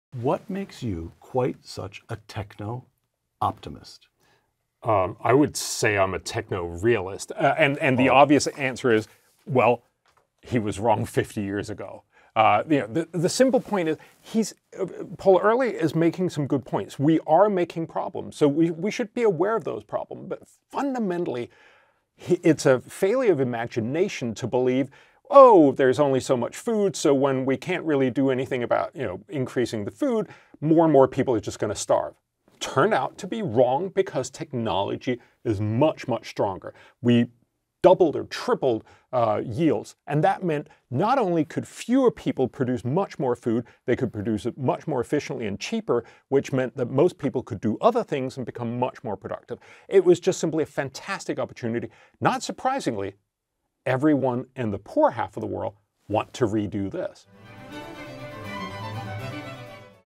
tunein to our next uncommonknowledge w/ Bjorn Lomborg discussing technology solving world problems
Dr. Lomborg discusses which policies we should prioritize and which we should pay less attention to or end. Lomborg also asserts the benefits of economic growth and says that by spending on technology, we can solve all kinds of big problems, including hunger.